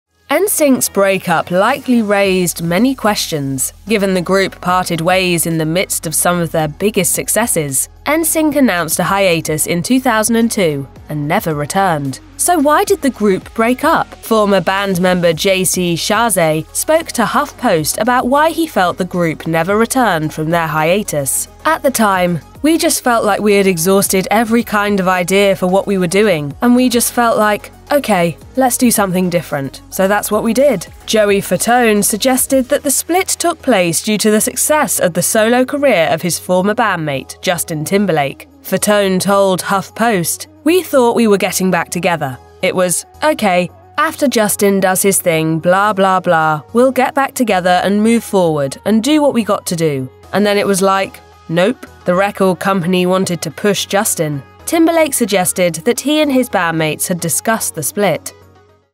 The Real Reason *NSYNC Broke Up
*NSYNC's breakup likely raised many questions, given the group parted ways in the midst of some of their biggest successes. It seemed each band member had their own story as to why they broke up.